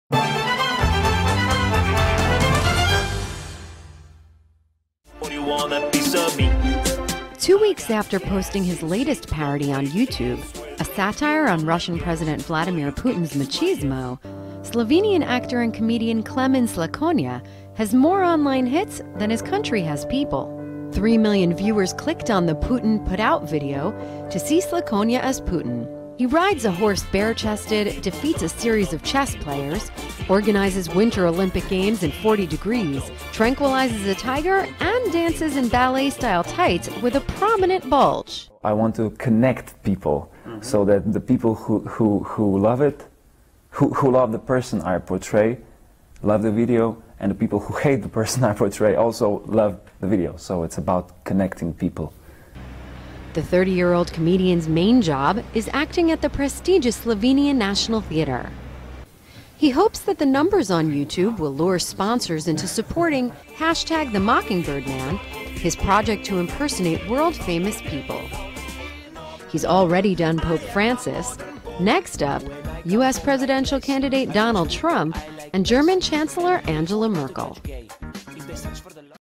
After Pope, now Putin parody for Slovenian comedian
A music video mocking Russian President Vladimir Putin goes viral and brings Internet fame to Slovenian comedian Klemen Slakonja.